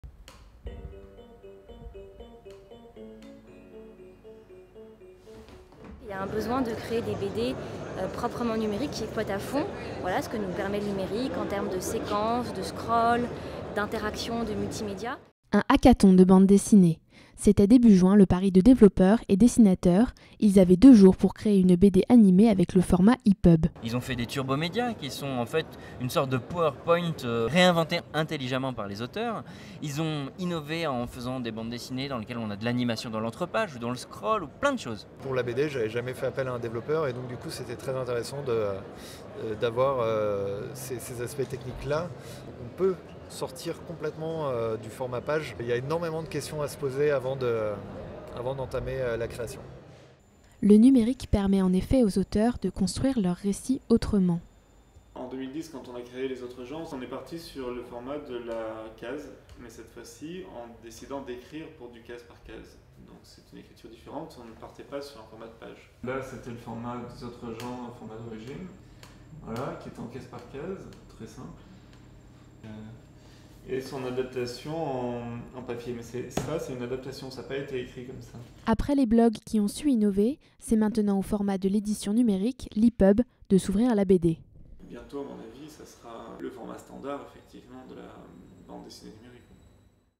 Le numérique, enjeu pour la BD / Digital comic strips are on the rise
Please turn the English subtitles on.
Video filmed, directed and edited by Pauline Bock.
Awarded for Google Empowering Students Scholarship.

Vidéo réalisée pour une bourse d'études
Reportage Pauline Bock